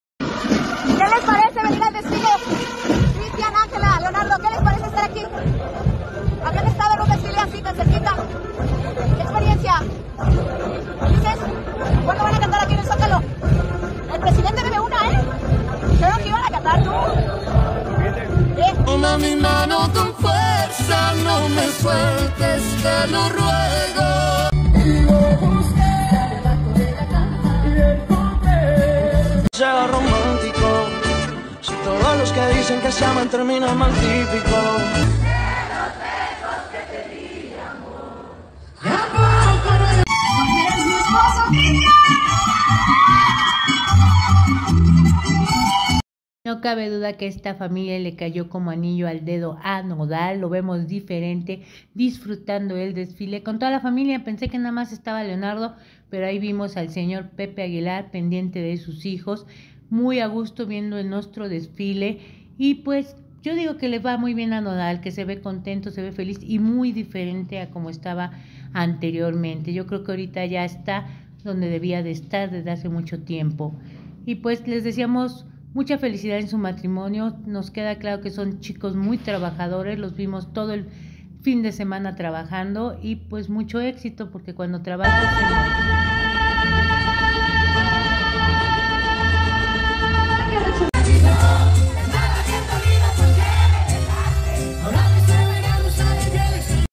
ANGELA Y NODAL DISFRUTAN DESFILE CON LA FAMILIA AGUILAR
Angela y Nodal disfrutando con la familia Aguilar el desfile